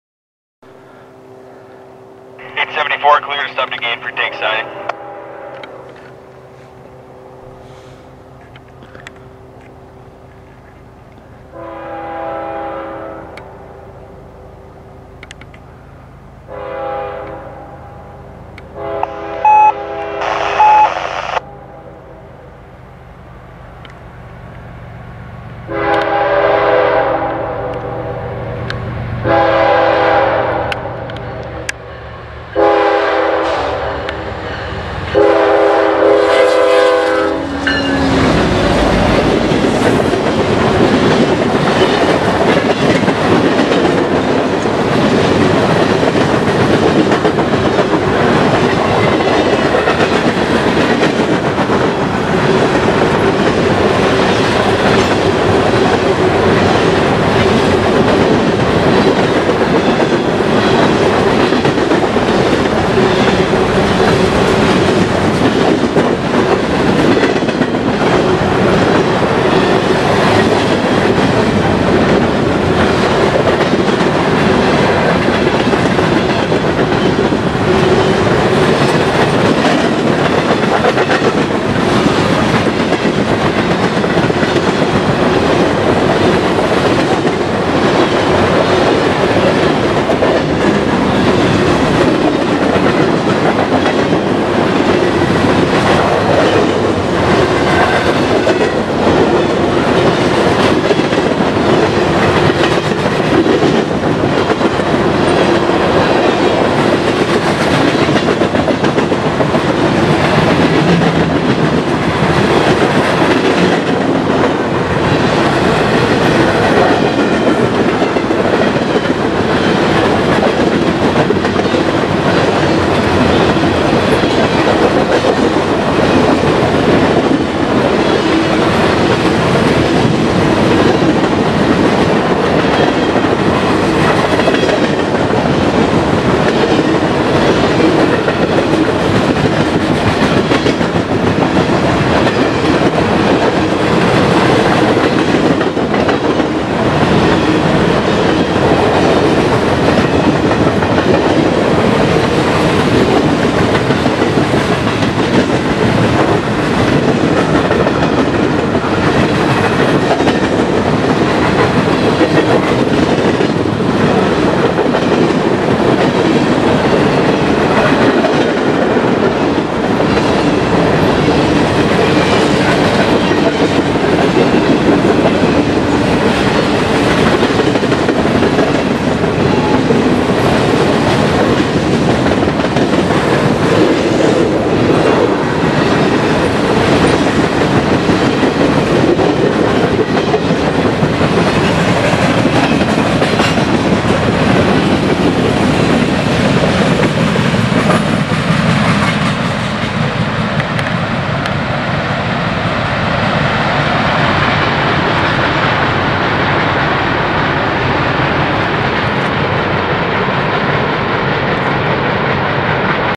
CN 874 (Empty Grain) @ MP 63.3 Edson Sub. 14OCT12 ES44DC 2285 Leading
Train: G874 51 (Vancouver BC To Edmonton AB)
Power: ES44DC 2285 SD70M-2 8871
Length:  174 Cars
Named Alberta Hoppers: Fort Saskatchewan, Fort Saskatchewan, Kitscoty, Barnwell, Alliance
Shot on October 14th 2012 @ 16:30